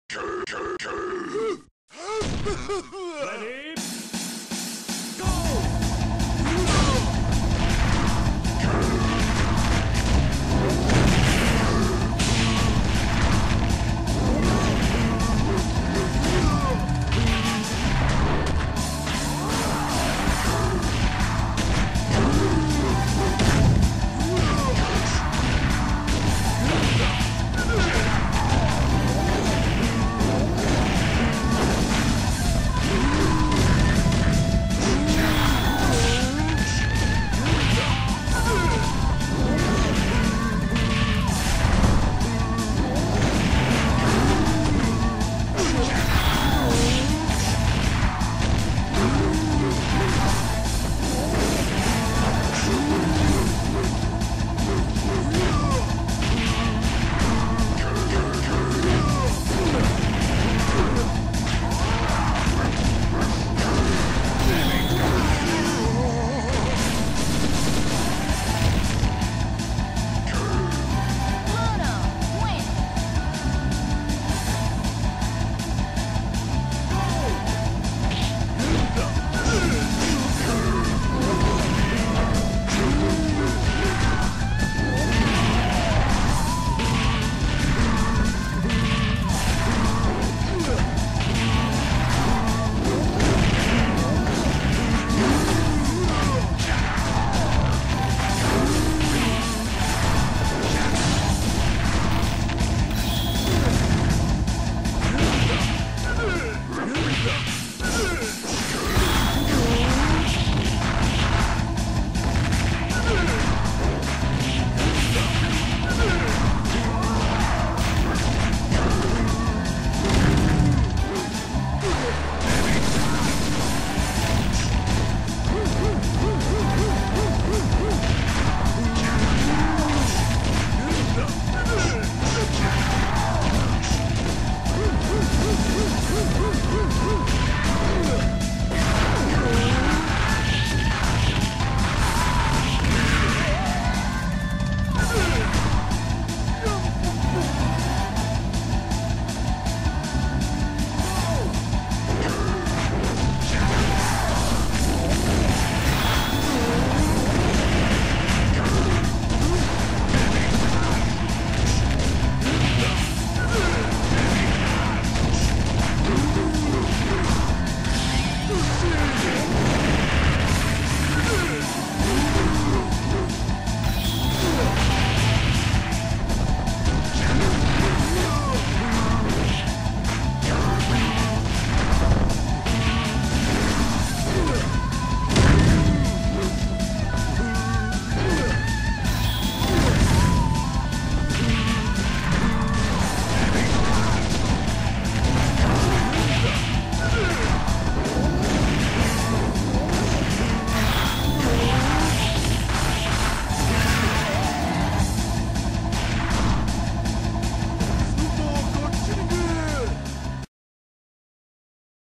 Chang vs Burnov
AI Match.

Chang Koehan By "M.M.R."
Burnov By "(^o^)"

Stage: Axel's Hideaway By Anti-11